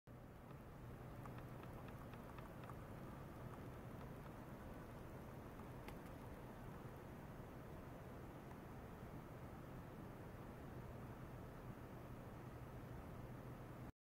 超音波の非線形現象をコントロールする実験（超音波システム研究所 ultrasonic-labo）
２台のファンクションジェネレータを利用することで
　２種類の異なる波形による（スイープ）発振により、

　１メガヘルツの超音波発振で、
　３００メガヘルツ以上の周波数変化を含めた

参考